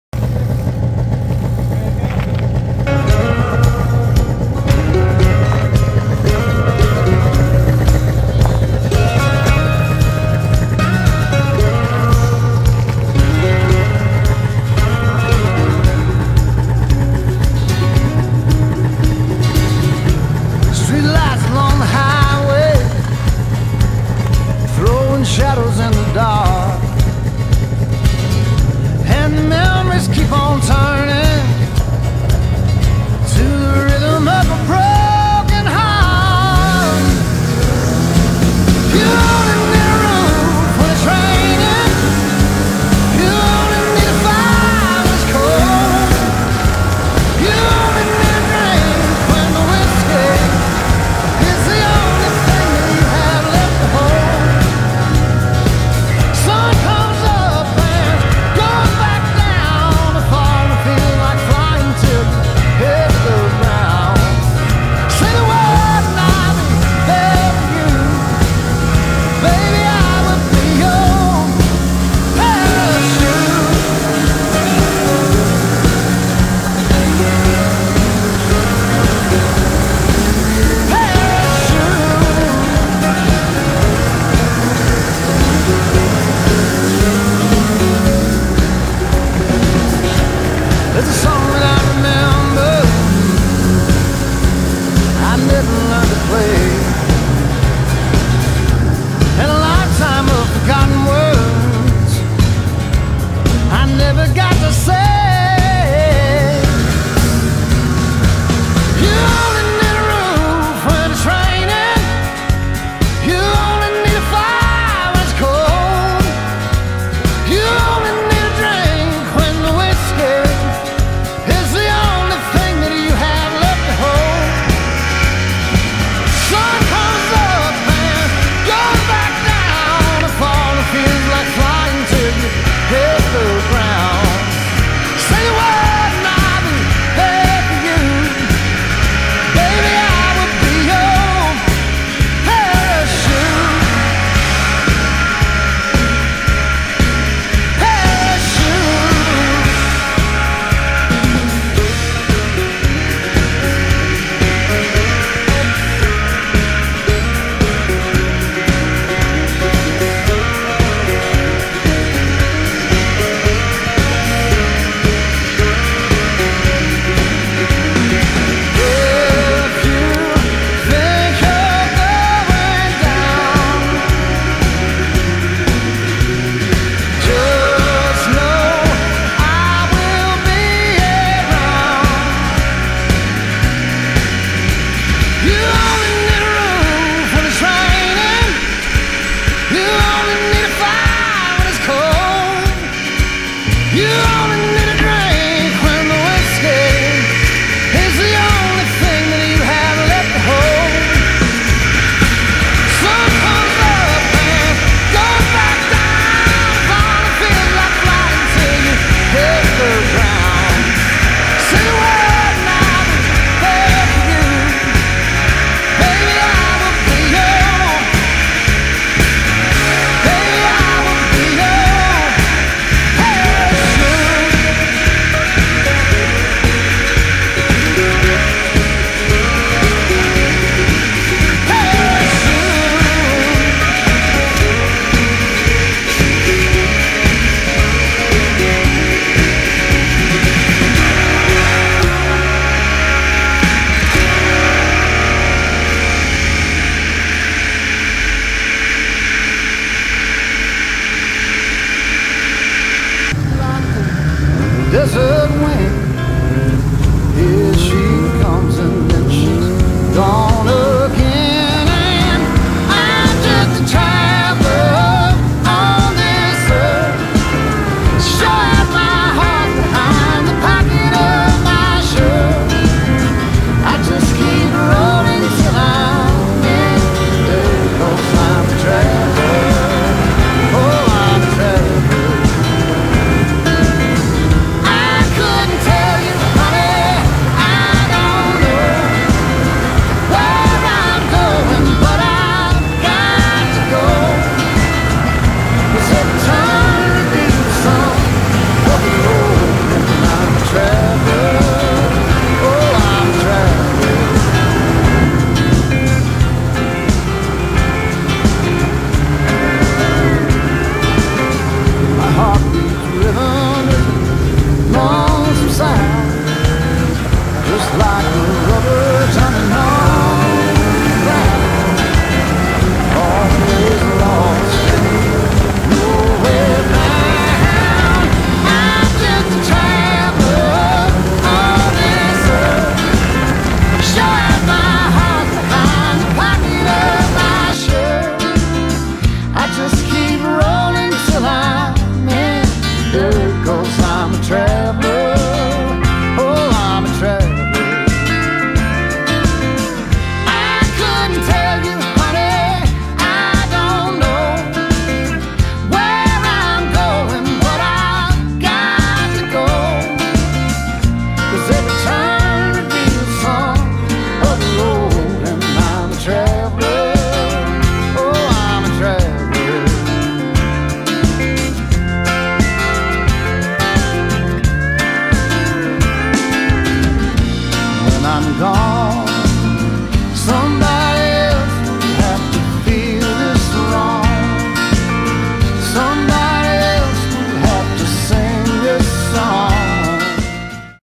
New Year's Snowmobile Trip
Snowmobiling, northern Wisconsin
2018 Polaris XCR 600, 2020 Polaris VR1 650 and 1997 Arctic Cat ZR 580